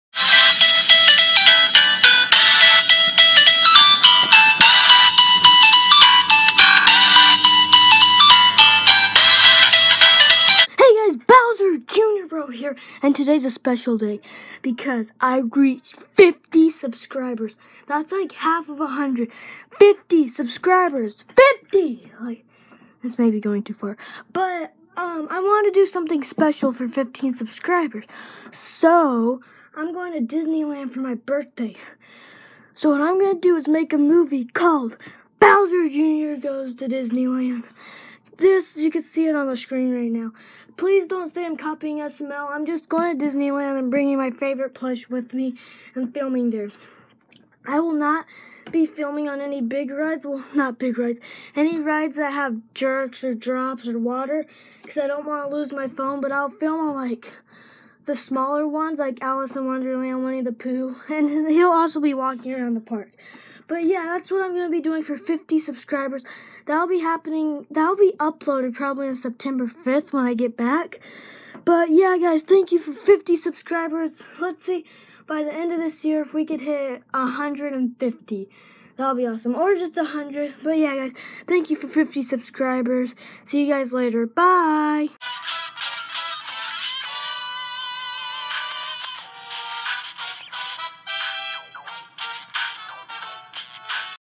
THX FOR 50 SUBSCRIBERS!!!
thank you guys so much i have only had this channel for 3 months and i got 51 subs thank you guys it means alot!